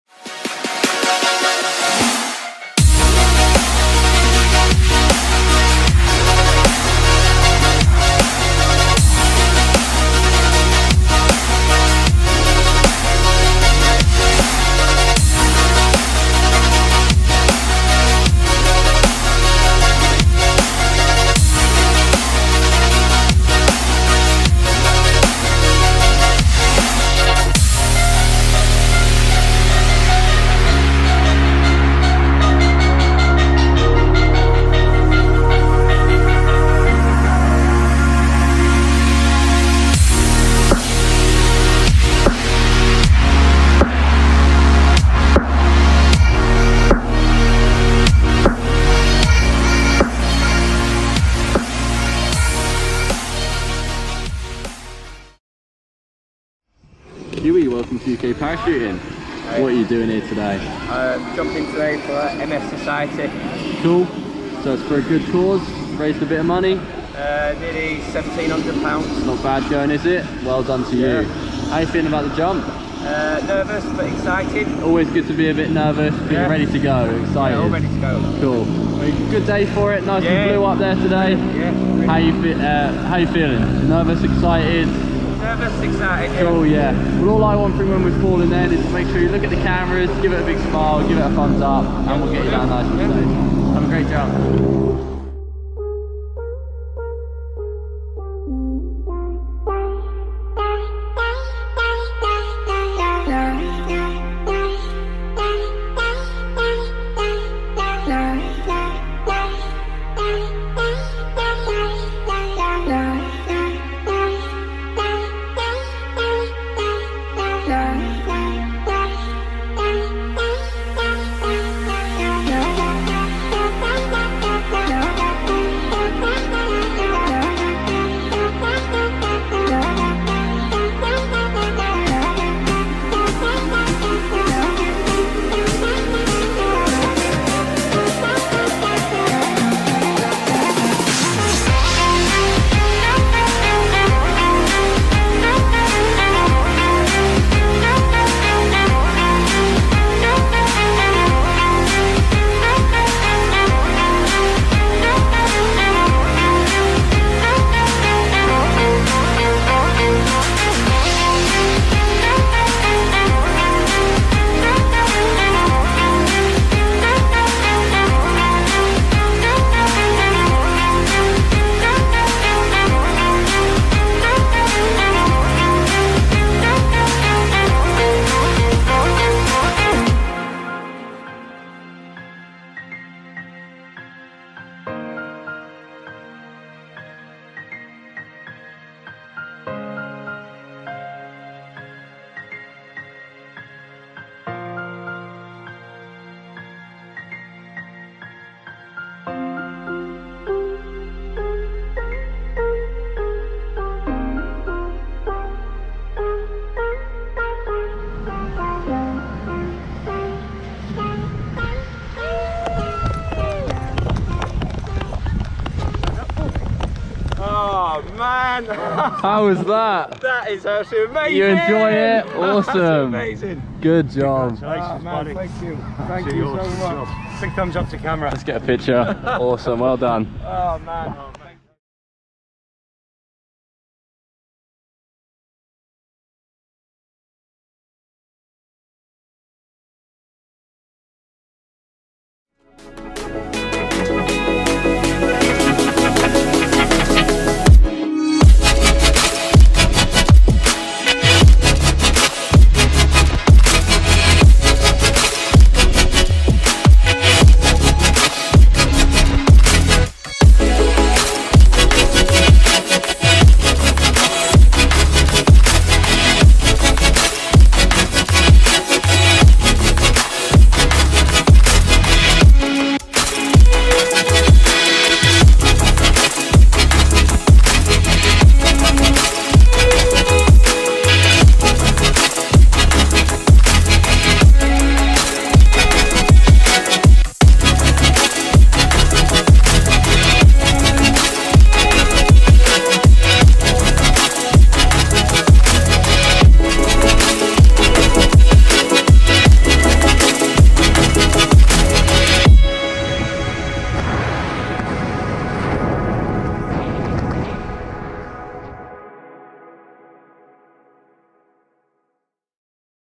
Tandem Skydive Beccles @Uk Parachuting
Multiple sclerosis Big Leap Skydive Challenge 1st August 2021.